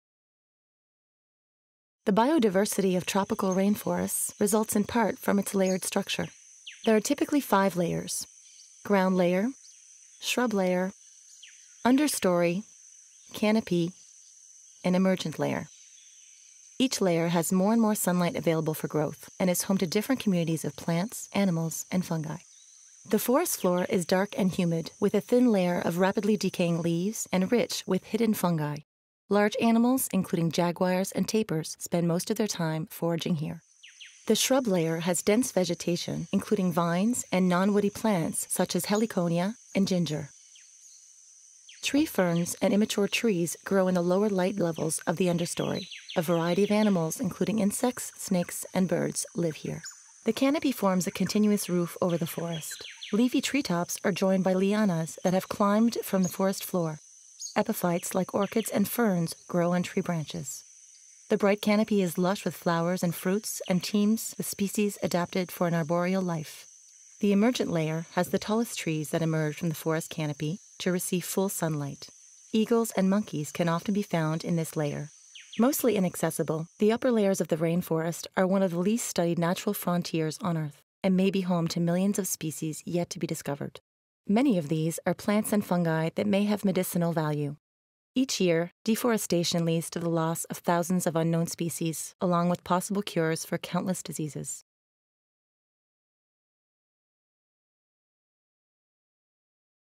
The Rainforest and Its Layers
A rainforest is a lot like a delicious parfait. They're both awesome to look at, great to have around, and they have lots of different layers!

As you travel from the ground upwards through the lush plant communities of a rainforest, you will encounter several distinct ecosystems, each with different groups of plants and animals that are specially adapted to surviving within that one particular band at that one particular height. This is one of the reasons biodiversity is so great in the rainforest-- life doesn't just spread out along the ground, it also spreads upwards with the shrubs and trees, using them like giant leafy apartment buildings!

For more videos about animals and the places they live,